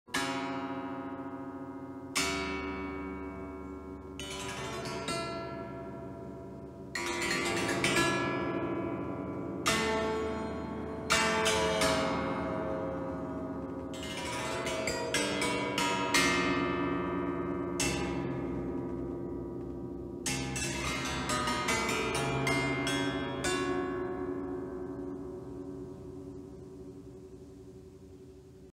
A brief performance on the randomly tuned tympanon
A brief performance on the randomly tuned tympanon by Scott F. Hall.